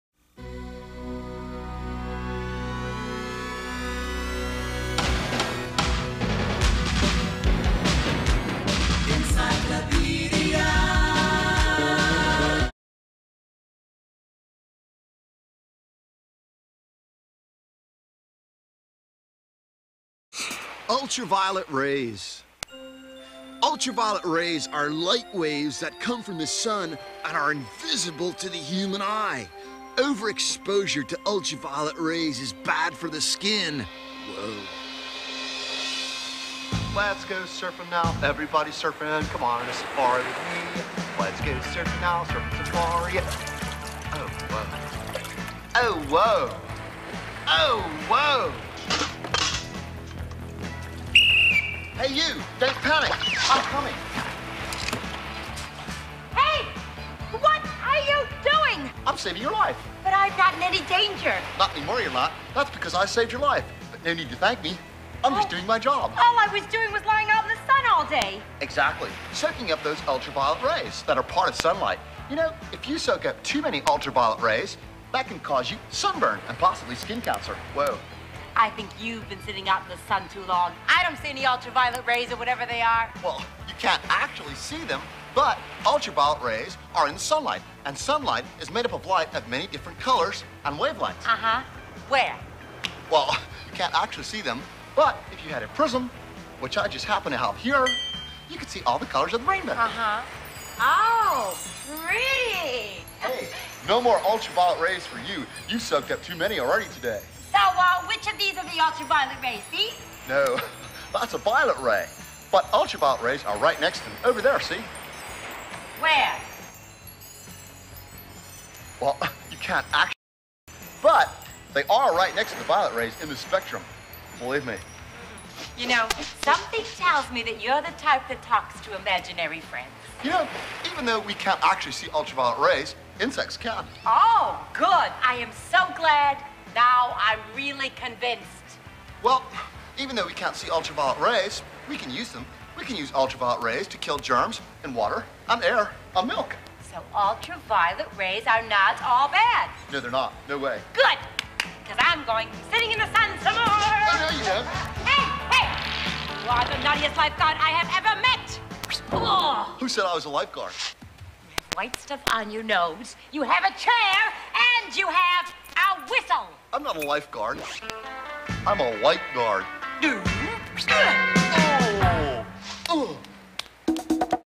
HBO CHILDREN'S TELEVISION WORKSHOP ULTRAVILOET RAYS
HBO CHILDREN'S TELEVISION WORKSHOP AWARD WINNING SKETCH COMEDY SEGMENT ABOUT ULTRAVIOLET RAYS